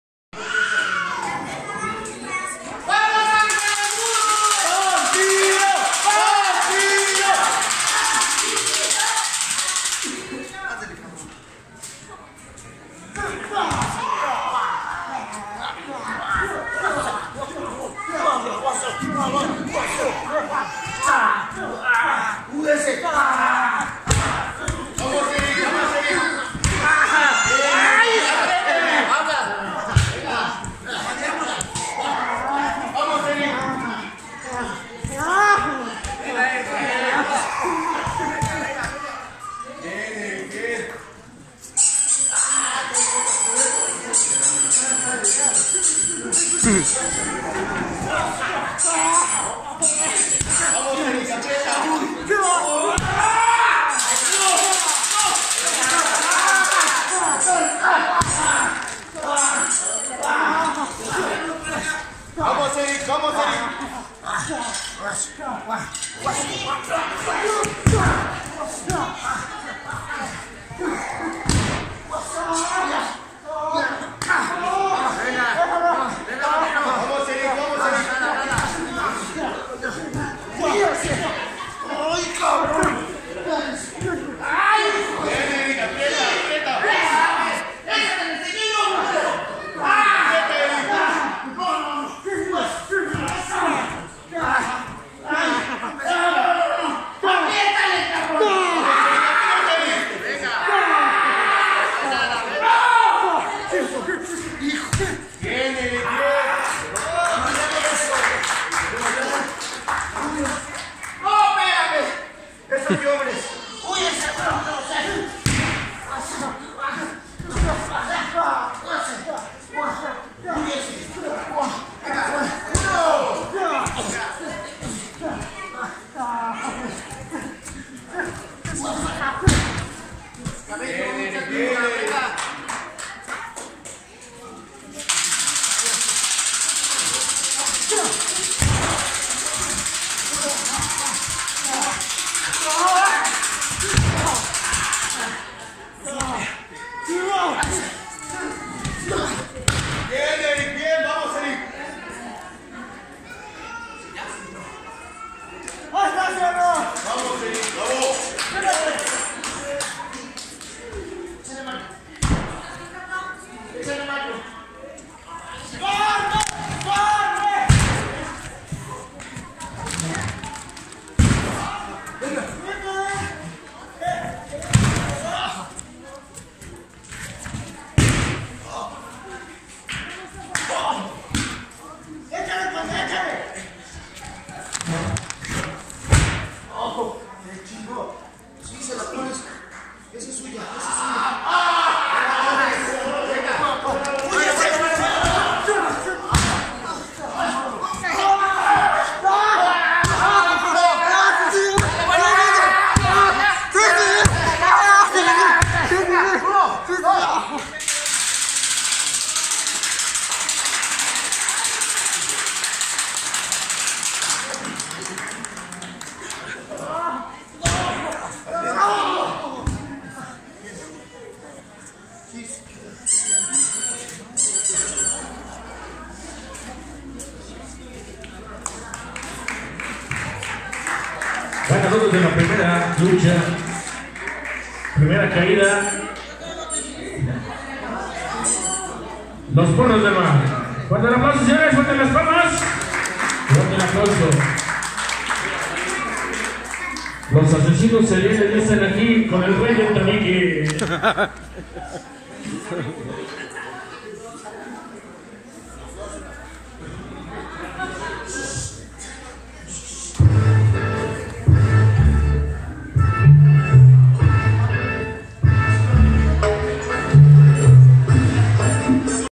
26 de diciembre de 2018
ANGEL O DEMONIO,LEÓN ROJO VS
CONDE BARTOK JR, ERIK EL VAMPIRO
ARENA COLISEO 23 DE JUNIO
FIN DE TEMPORADA 18 .23DIC18.
1ra caída.